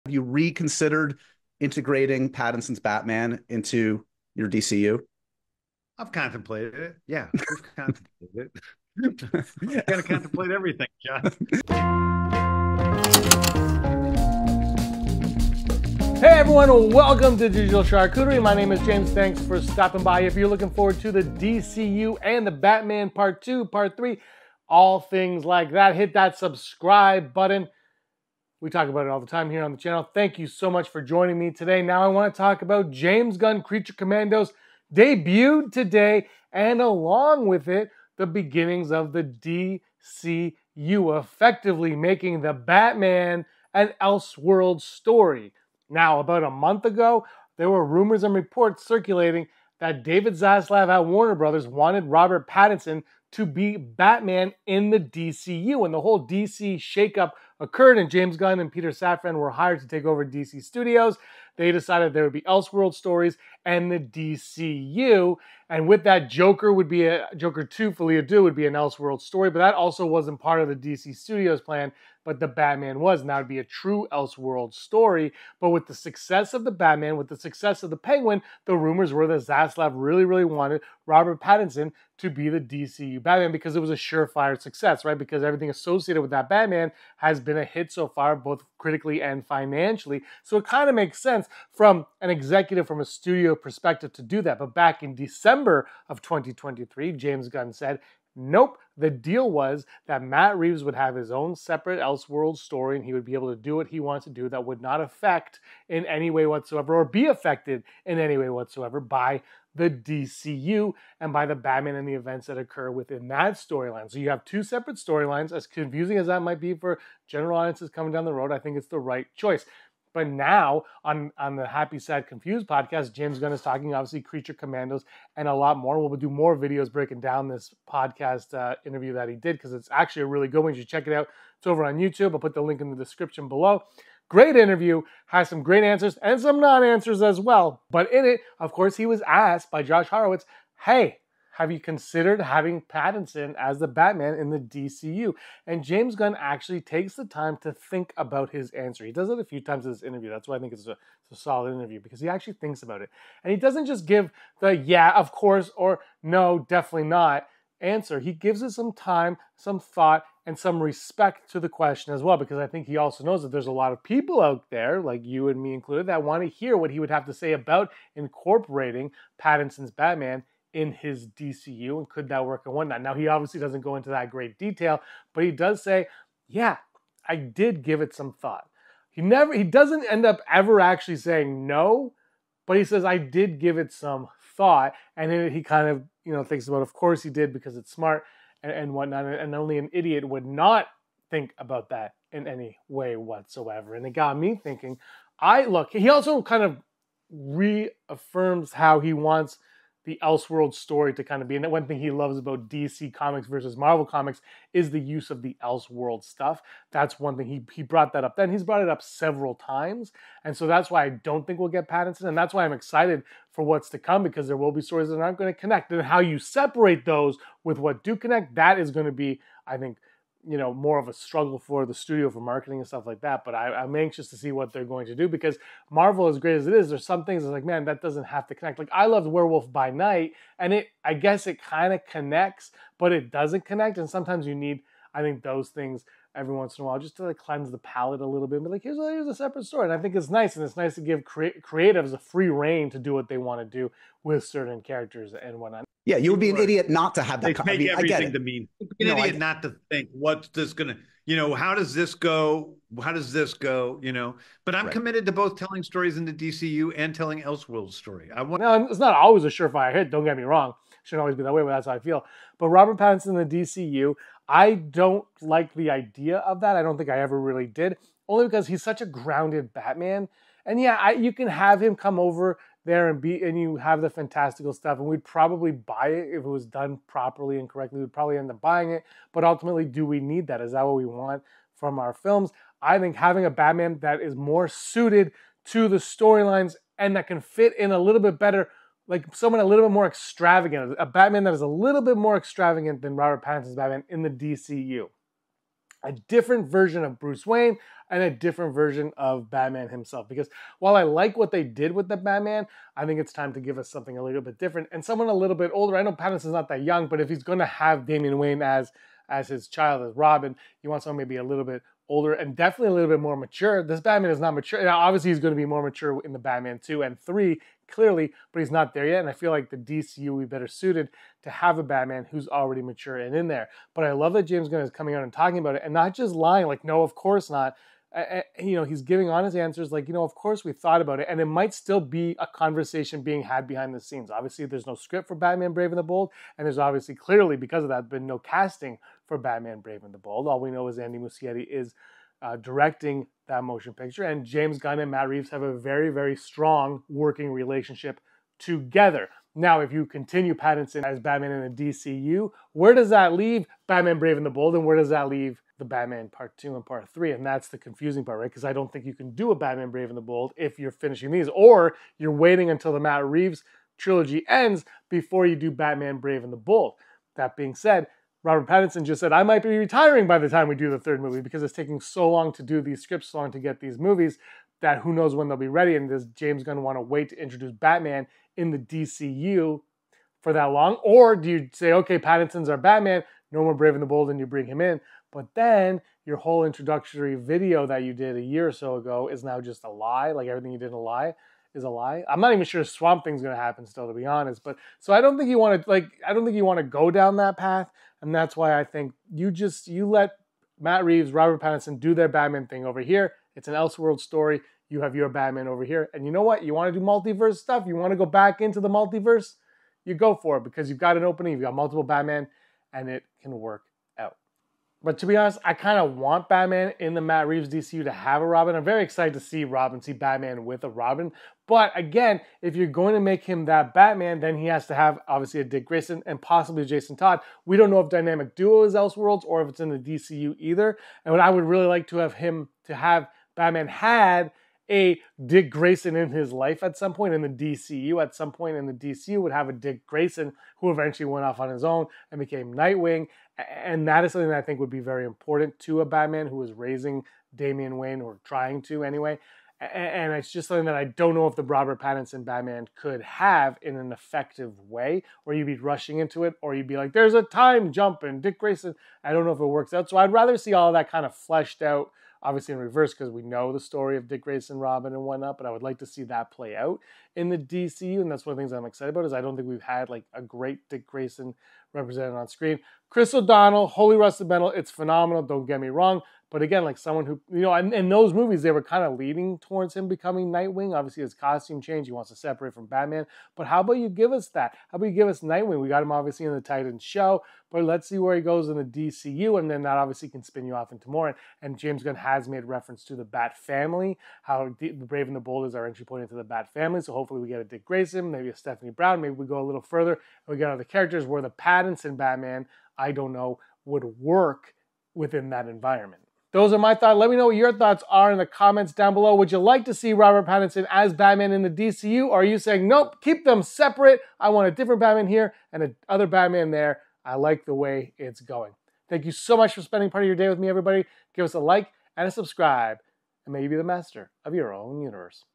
James Gunn CONSIDERING Robert Pattinson as the DCU Batman
Did James Gunn consider Robert Pattinson’s Batman for the DCU?  In a recent appearance on the Happy Sad Confused podcast, Gunn opened up about the possibility of Pattinson’s Dark Knight joining the main DCU timeline. However, he explained why Elseworlds projects, like Matt Reeves’ The Batman universe, allow for more creative freedom and storytelling depth.

Could Pattinson ever cross over into the DCU? What does this mean for the future of Batman under Gunn’s vision? And how does this decision impact upcoming DCU projects like Superman: Legacy and The Brave and the Bold?

Watch as we dive deep into Gunn’s comments, break down his vision for the DCU, and explore what Elseworlds could mean for fans of Batman and beyond.